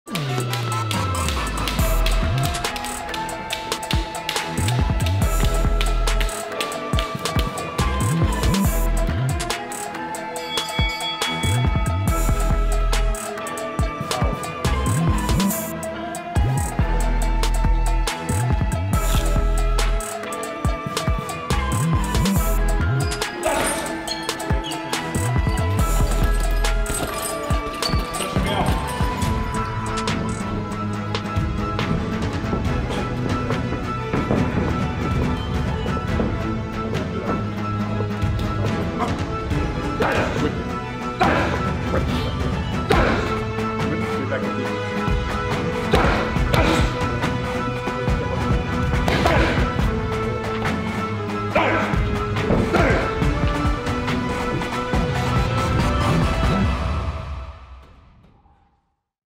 How Chinese heavyweight boxer, Zhilei Zhang trains for a big fight
Ever wondered how an international heavyweight boxer trains before a big fight?

We caught up with Chinese heavyweight boxer Zhilei Zhang to show you exactly how…